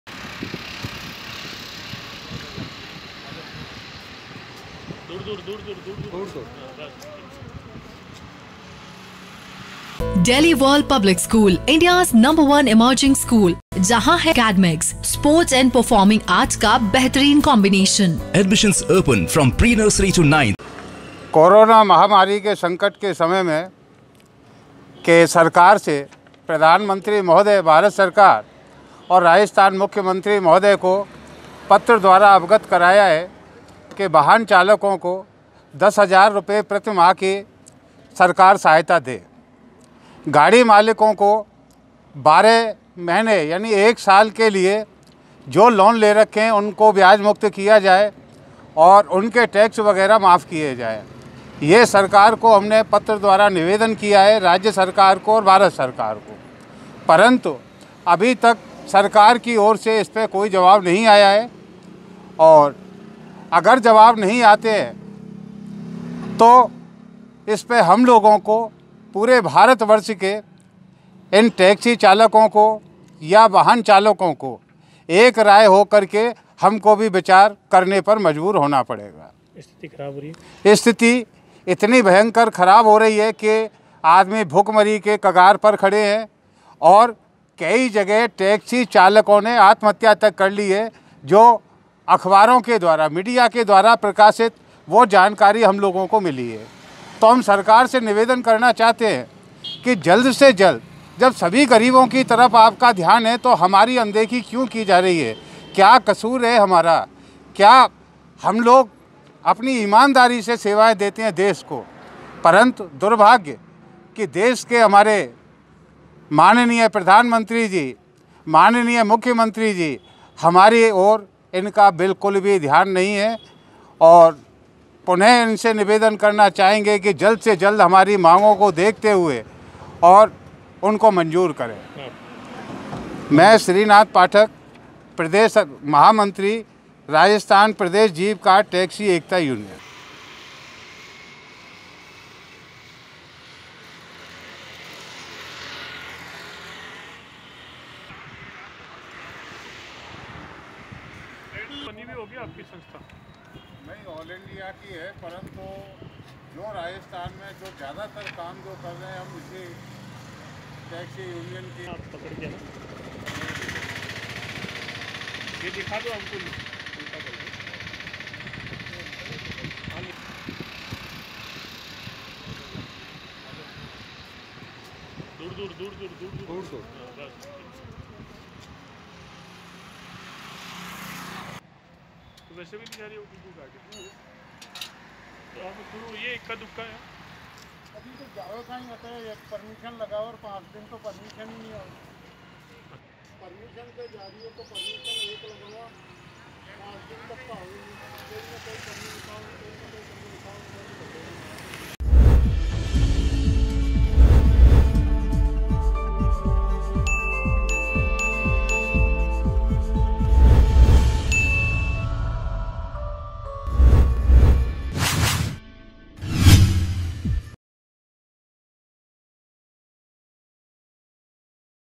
AJMER NEWS | जीप कार टैक्सी एकता यूनियन ने मुख्यमंत्री और प्रधानमंत्री को भेजा ज्ञापन | MTTV INDIA
जीप/ कार टैक्सी एकता यूनियन ने मुख्यमंत्री और प्रधानमंत्री को भेजा ज्ञापन
कोरोना महामारी की वजह से टैक्सी चालकों का व्यापार हुआ ठप
कोरोना महामारी के संकट में टैक्सी चालकों को अब मदद की जरूरत
चालक व वाहन मालिक की आजीविका हेतु 10-10 हजार के मासिक सहायता राशि की मांग